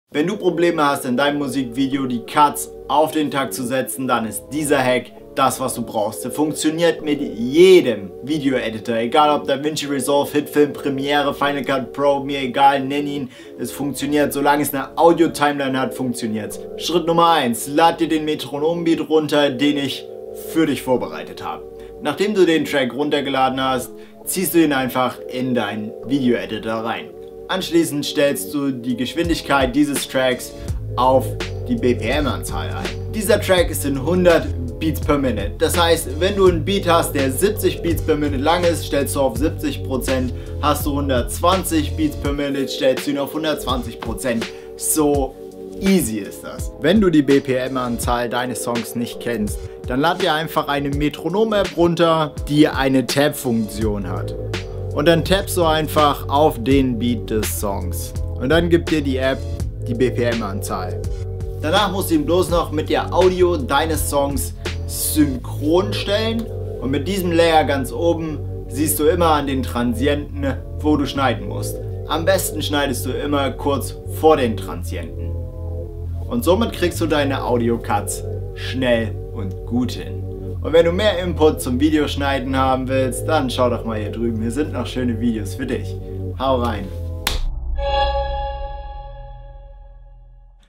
Video auf den Beat / Takt Schneiden - Easy für Anfänger - Musikvideo Tipp
Wenn du bein deinem Musikvideo auf den Beat schneiden möchtest dann ist dieser Tipp der beste den du nutzen kannst. Gerade als Anfänger im Video - schneiden Ist das einer der Besten Mittel deine Cuts auf den Takt zu bringen

Ich wünsche dir Viel Spass beim Videos schneiden!